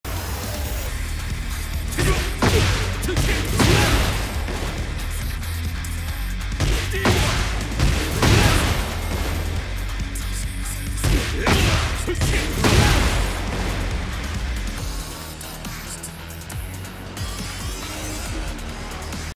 Really easy Jin 10 frame punish string (2,4,F,1,2) (For beginners)
Made this video to teach some friends of mine how to play Jin, I also figured I'd make it public for anyone else trying to learn Jin.